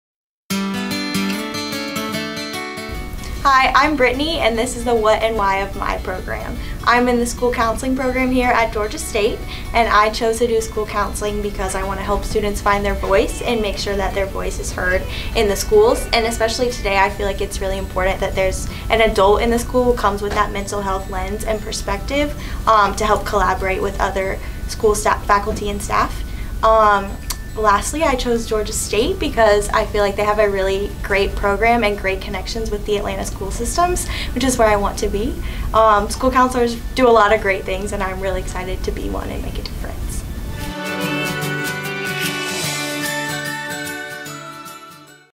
School Counseling M.Ed. - Brittney Norment
Find out what brought graduate student, Moya Grant, to Georgia State University to study School Counseling. Learn more about our School Counseling M.Ed. program